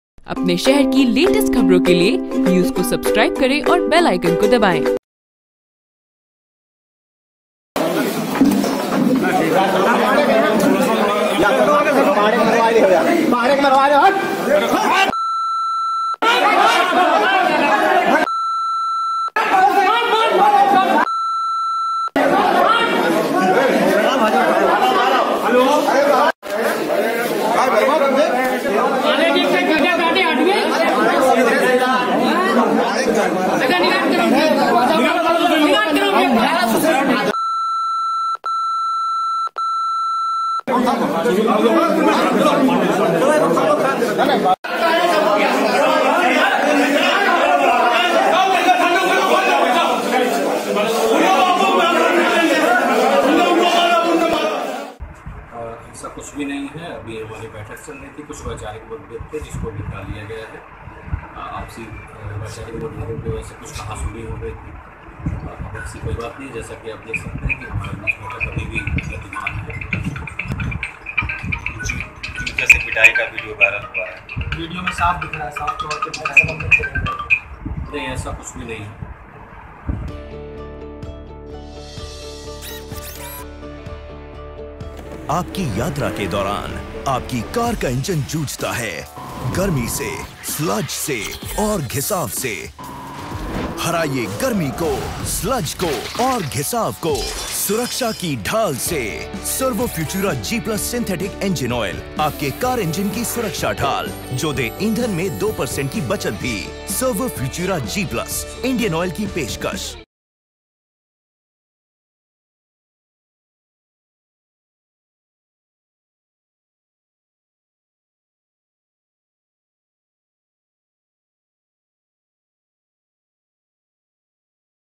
Hardoi: BSA ऑफिस बना अखाड़ा, शिक्षा अधिकारियों के बीच हुई जमकर जूतेबाजी
हरदोई के बेसिक शिक्षा अधिकारी की मौजूदगी में चल रही खंड शिक्षा अधिकारियों की मीटिंग में जमकर जूतेबाजी हुई.. खंड शिक्षा अधिकारियों में बीएसए के कमरे के अंदर ही जूता चलने लगा.. कमरे के अंदर अचानक हंगामा देखकर लोगों की भीड़ जमा हो गई.. वहीं जूतेबाजी का वीडियो सोशल मीडिया पर वायरल हो गया है.